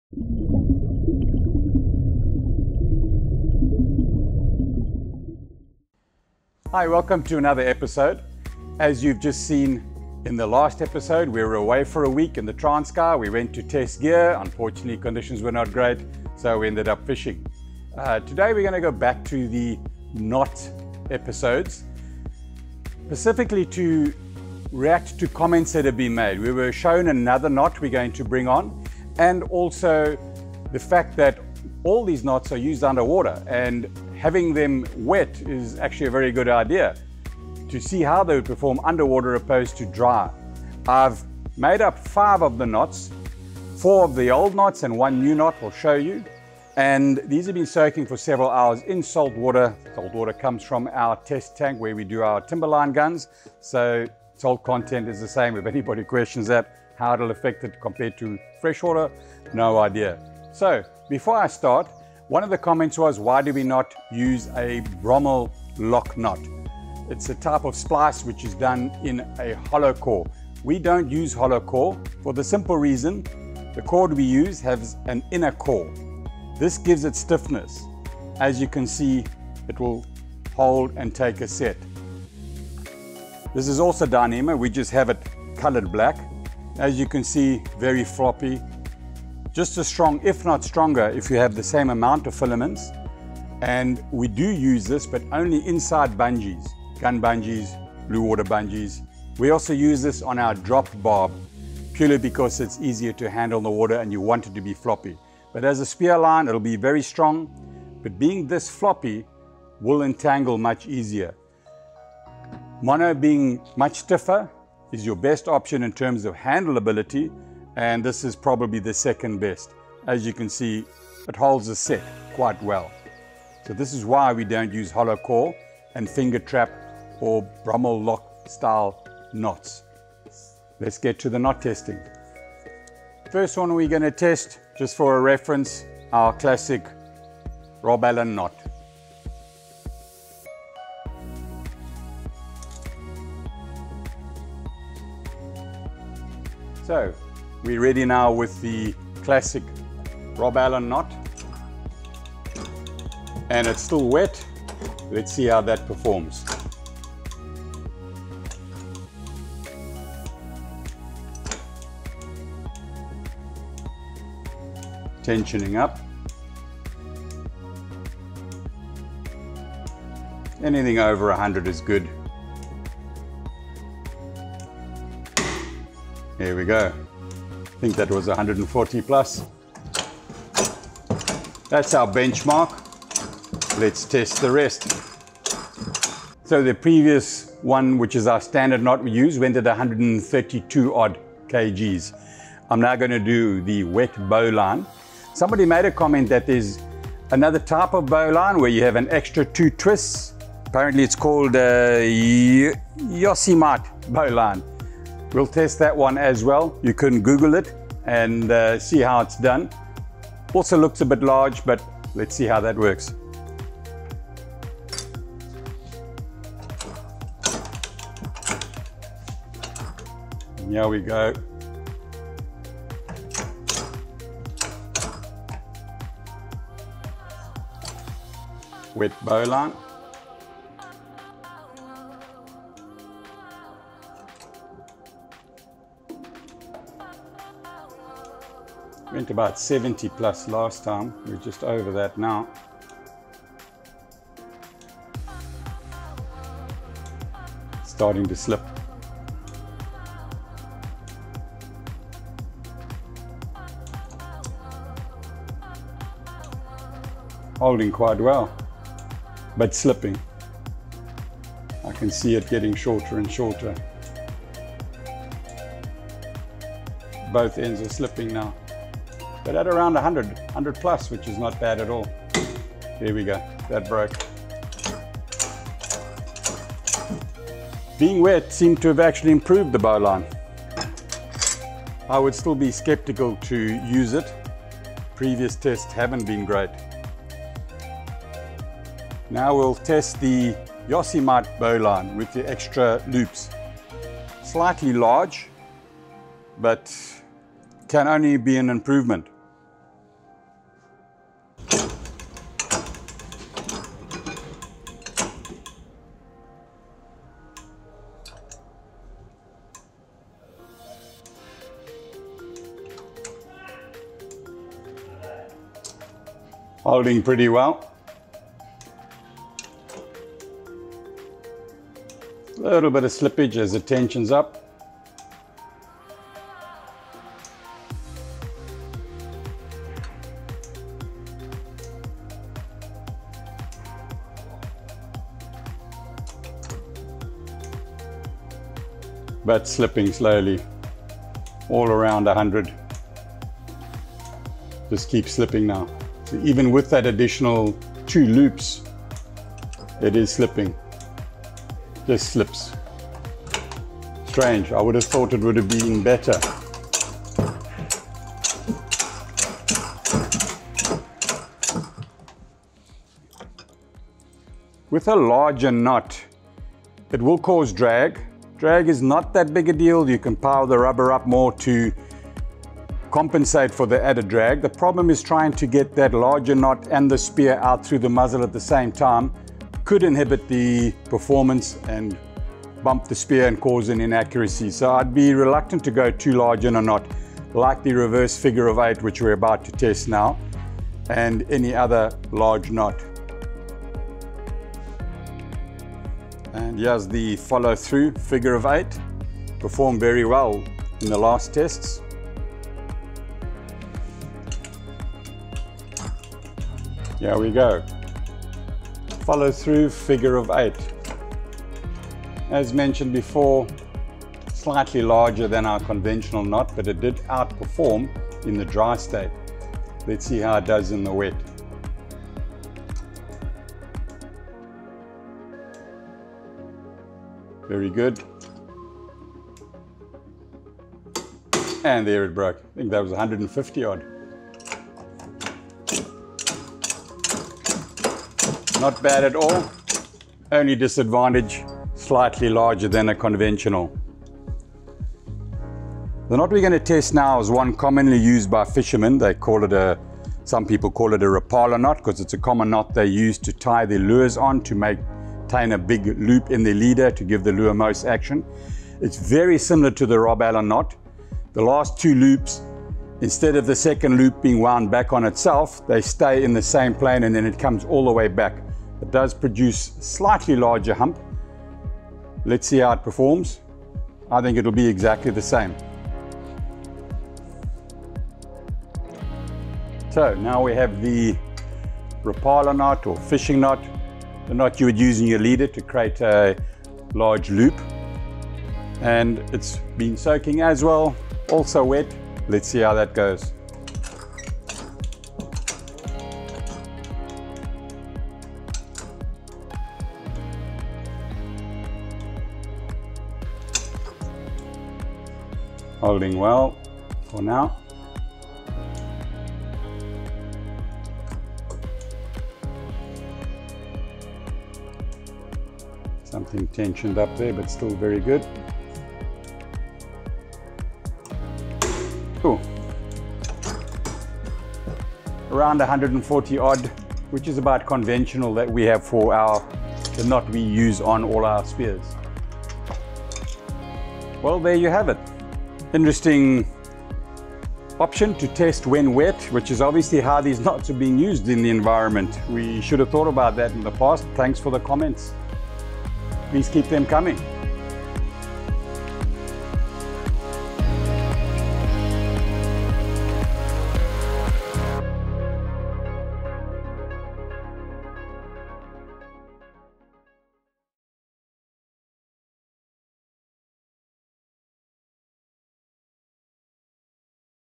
Rob Allen | Testing Knots When Wet | 2022
In this specific video, we test the previous knots but this time wet. Scientifically to get the most accurate results we would have to do at least 30 repeated tests of each knot minimum to find the average result.

If you would like to learn how to do these knots you can have a look at these links.

Chapters:
00:00 – 00:59 – Introduction
01:00 – 02:10 – Why we don’t use the Brummel Lock Knot
02:11 – 03:05 – Rob Allen/Standard Conventional Knot
03:06 – 04:48 – Bowline Knot
04:49 – 06:33 – Yosemite Knot
06:34 – 07:24 – Figure 8 Follow-through Knot
07:25 – 08:52 – Rapala Knot
08:53 – 09:07 – Conclusion
09:08 – 09:28 – Outro